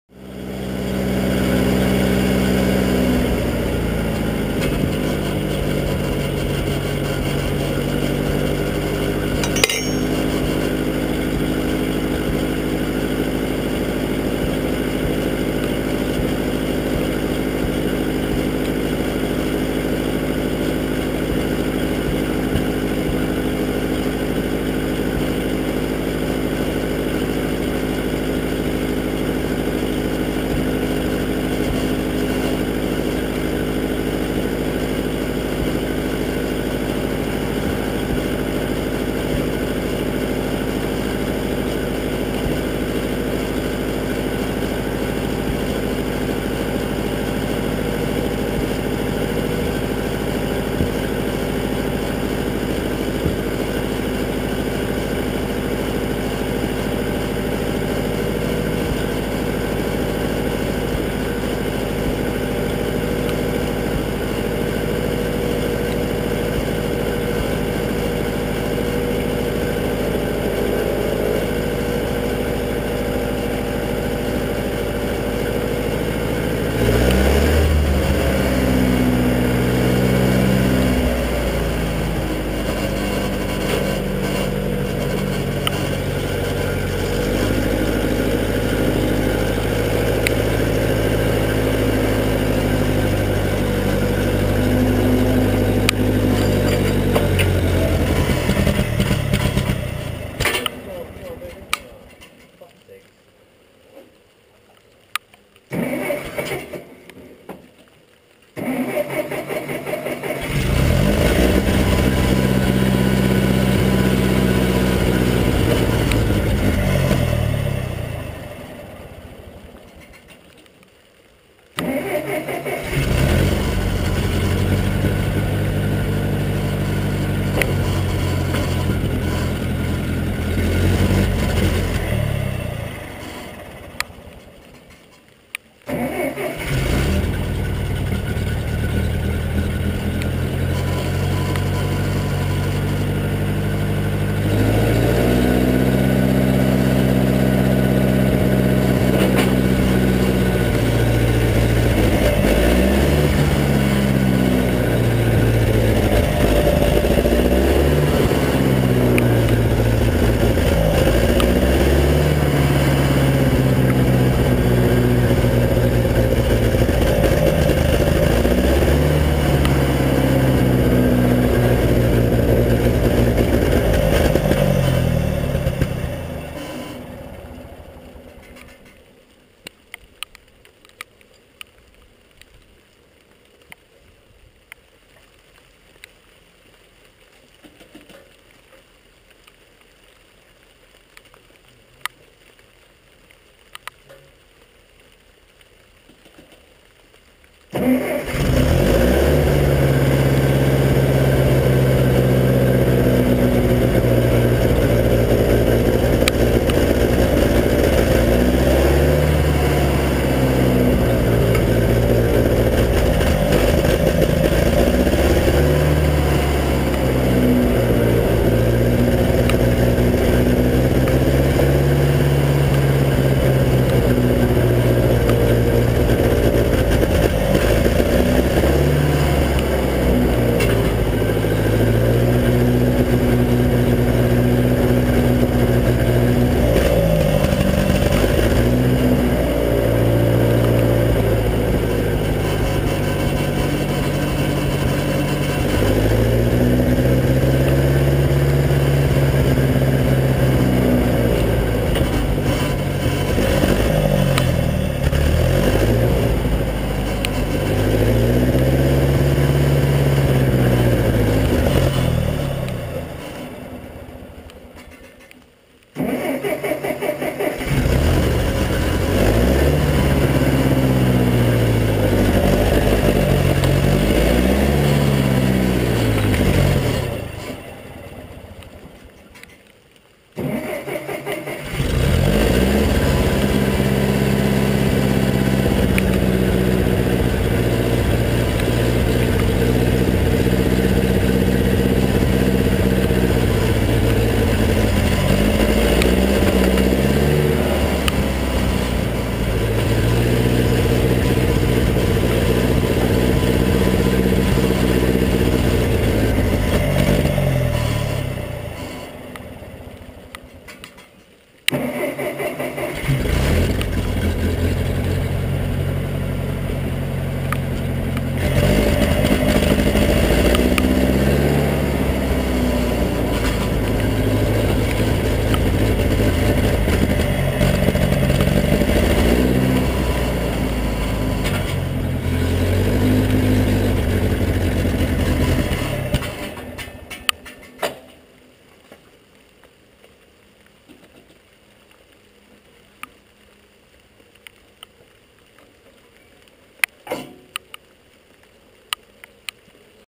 MF 202 workbull stalling under load
Trying to figure this out